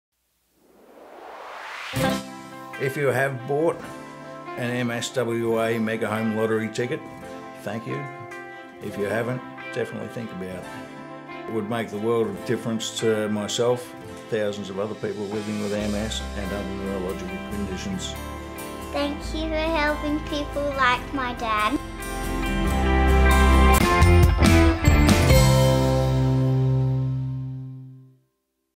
Thank you from MSWA Member Matt Nichols and daughter Willow Nichols
The MSWA Mega Home Lottery is our biggest fundraiser. Thanks to your generosity, MSWA is able to provide vital healthcare services, high support accommodation and is a leading contributor towards research for people living with MS and all neurological conditions.